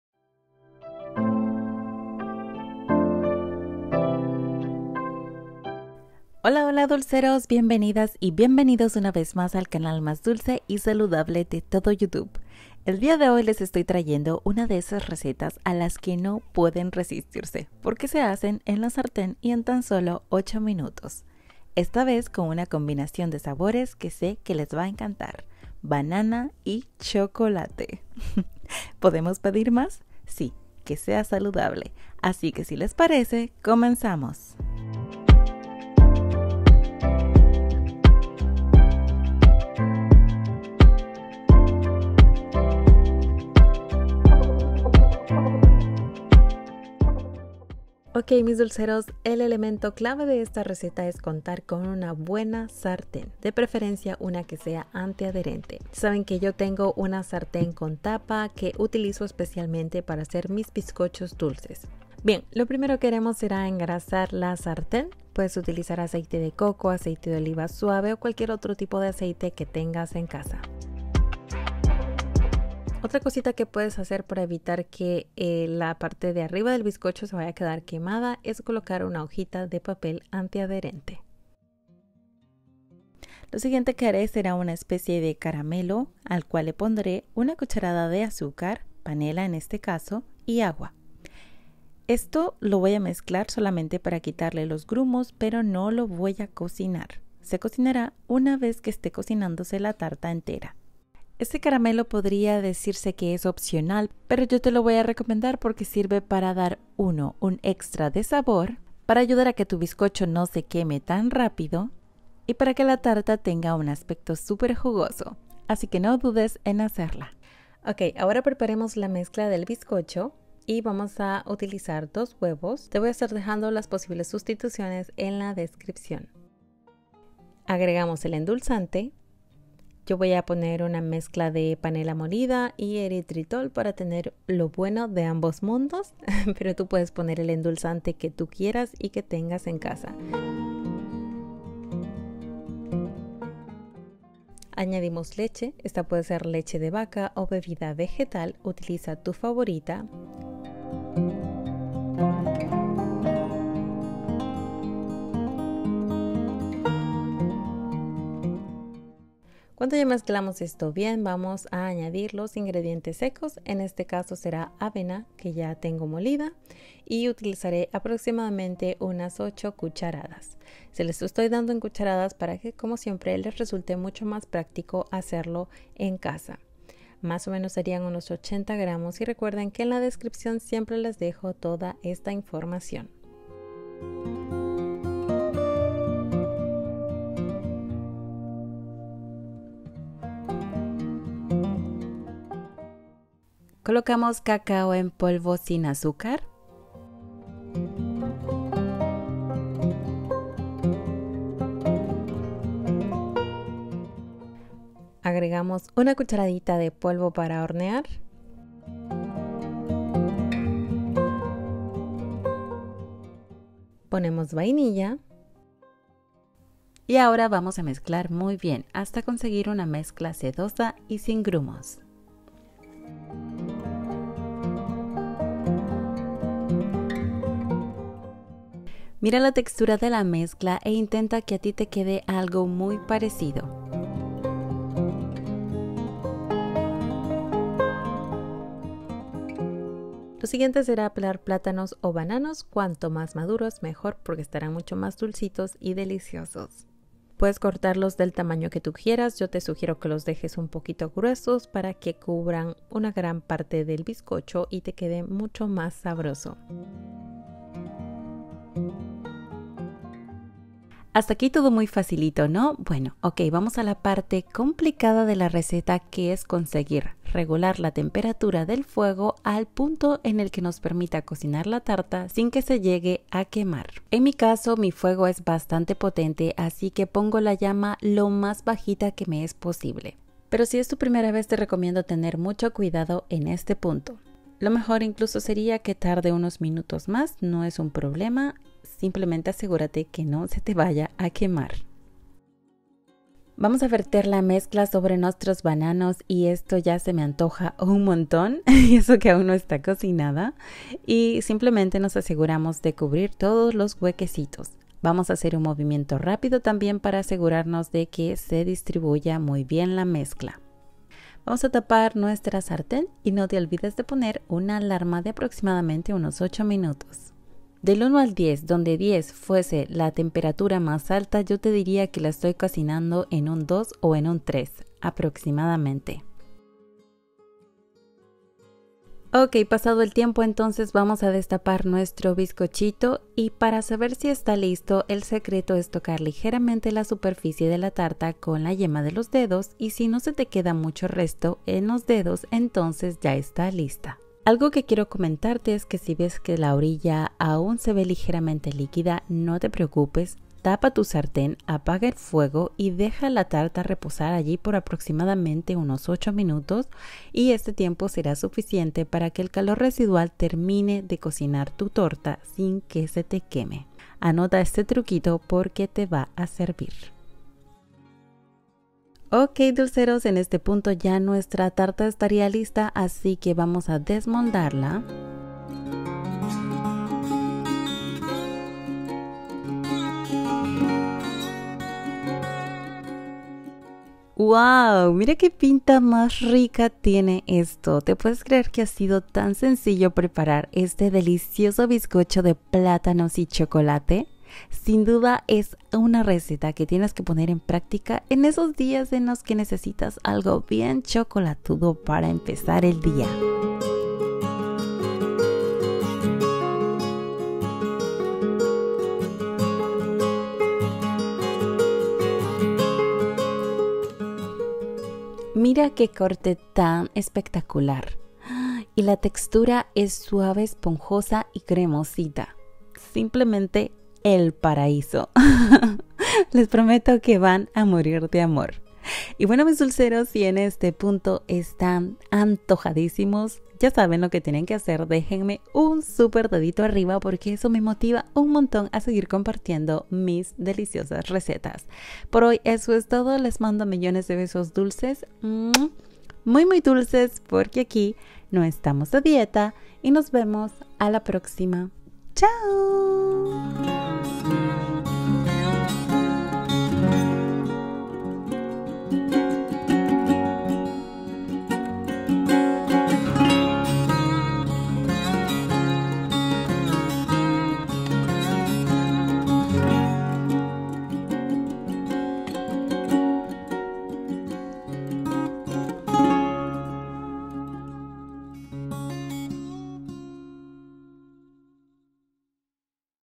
Si tienes Plátanos, cacao y 8 minutos prepara este delicioso postre Saludable | Auxy
❤ Descarga mis LIBROS (e-Books) de cocina saludables en tu móvil, tablet o laptop. Descarga instantánea aquí: 🔴
Gracias por apoyar mis investigaciones para crear más recetas saludables!
Hola hola mis dulces seres!!

Ingredientes para una tarta de 18 cm

🍌Caramelo: 1 cda de panela y 1 cda de agua
🍌4 bananas medianas
🍌2 huevos
🍌3 cdas de endulzante con o sin calorías
🍌3 cdas de leche de vaca o vegetal
🍌8 cdas | 80 g de avena molida u otra harina saludable
🍌2 cdas de cacao sin azúcar
🍌1 cdita de polvo para hornear
🍌 1 cdita de vanilla

Cocción:
🔥 Fuego bajito por 8-12 min con la sartén tapada
🔥 Horno 180ºc por 12-15 min

📌 Se conserva en la nevera hasta por 1 semana, así que es ideal como desayuno para ahorrar tiempo.

besitos dulces, muy muy dulces porque aquí no estamos a dieta y nos vemos a la próxima!
*No dejes de comer, aprende a elegir!

Auxy